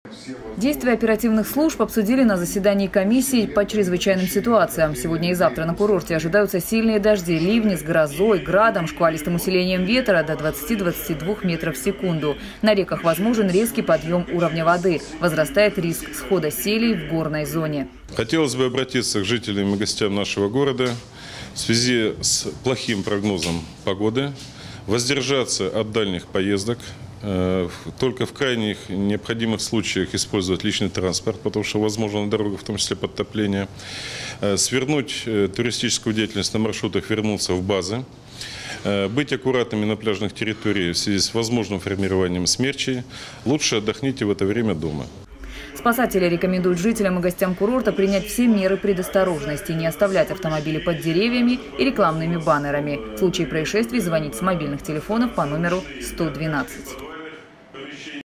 Все службы Сочи переведены на усиленный режим работы. Новости Эфкате
В городе продолжает действовать штормовое предупреждение.

Действия оперативных служб обсудили на заседании комиссии по чрезвычайным ситуациям. Сегодня и завтра на курорте ожидаются сильные дожди, ливни с грозой, градом, шквалистым усилением ветра до 20- 22 м/с. На реках возможен резкий подъём уровня воды. Возрастает риск схода селей в горной зоне.